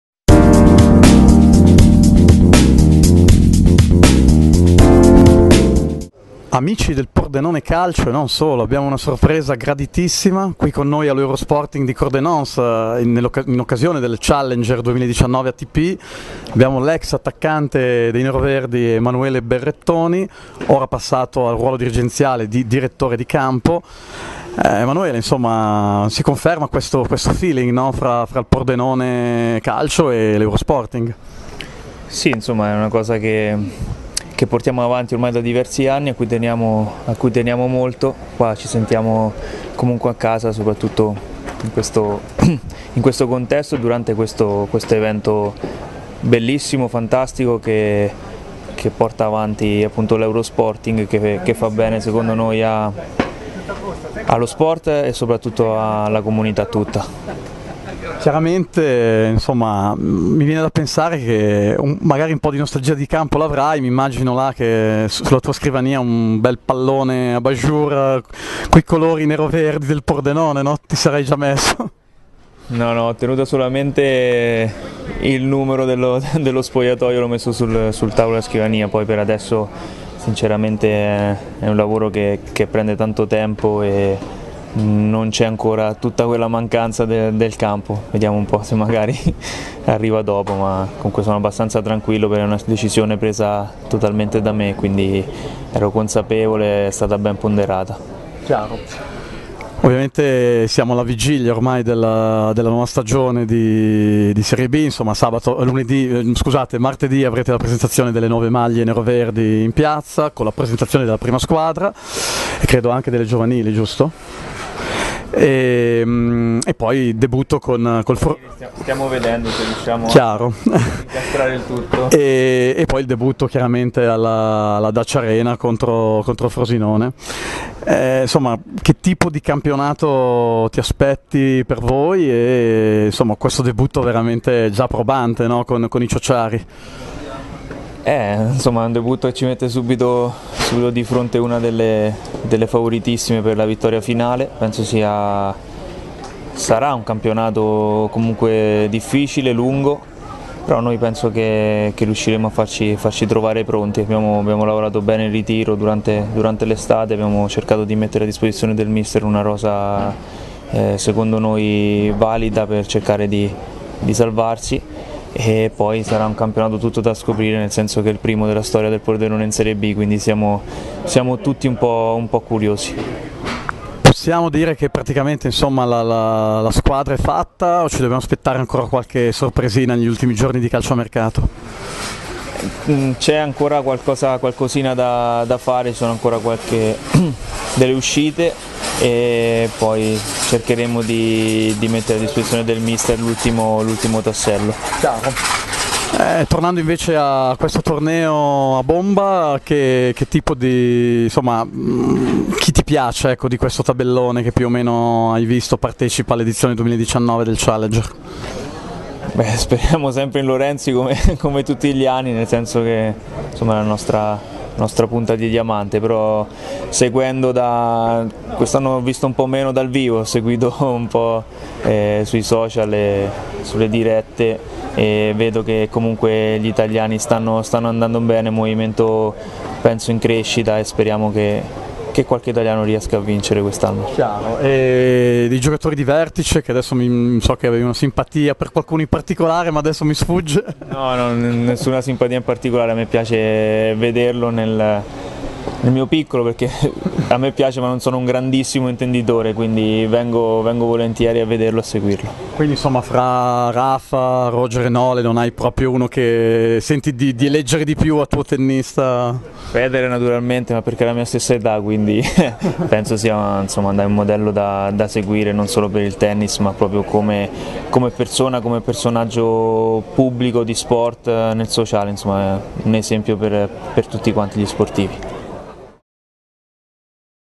EMANUELE BERRETTONI - Acqua Dolomia Serena Wines Tennis Cup
DolomiaSerenaTennisCup - L'intervista a EMANUELE BERRETTONI, direttore di campo del Pordenone Calcio, realizzata in occasione del Torneo ATPChallenger internazionale FVG in corso sui campi dell'Eurosporting Cordenons.